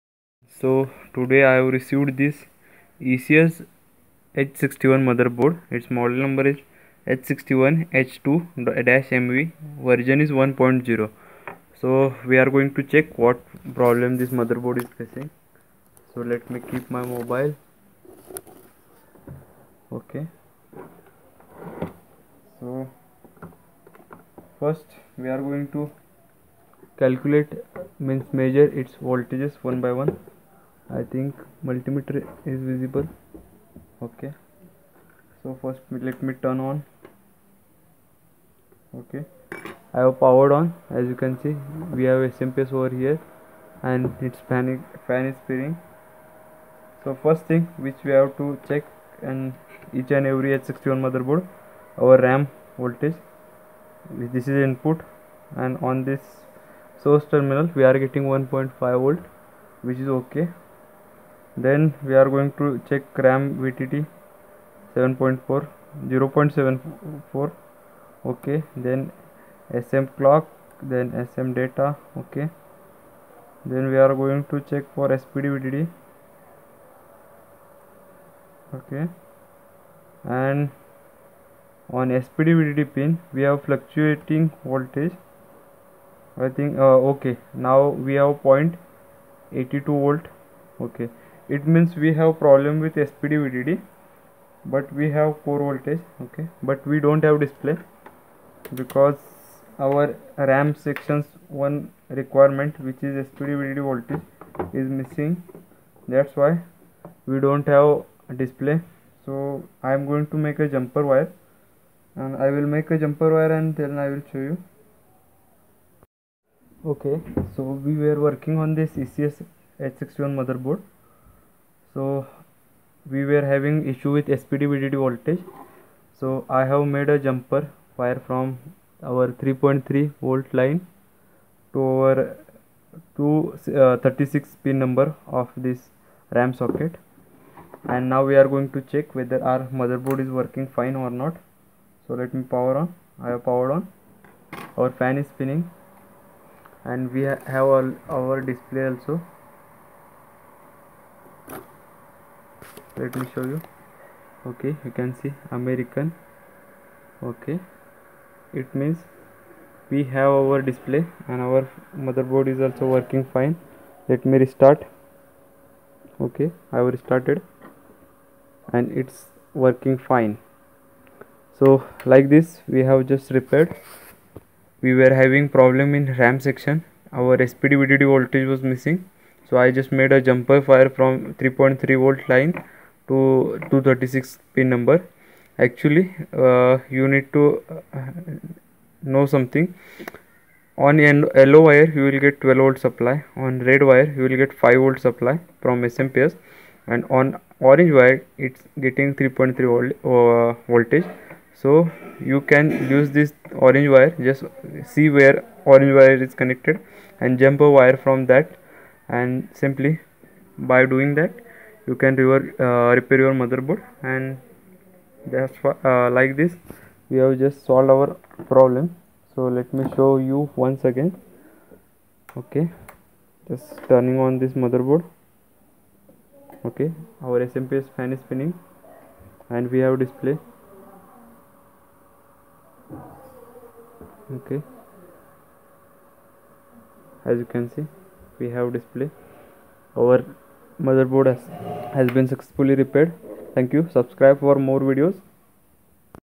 ECS H61 No Display issue SPD_VDD Missing
Today i have recived ECS H61 which was having No display issue

So first step is to check ram section
1 check ram voltage 1.5v ok
2 check ram vtt voltage 0.75v ok
3 check smclk voltage 3.3v ok
4 check smdata voltage 3.3v ok
5 check spdvdd voltage it should be 3.3v but in this board it was missing

so is spdvdd is missing then you can make a jumper from 3.3v line to spdvdd pin

now board is working fine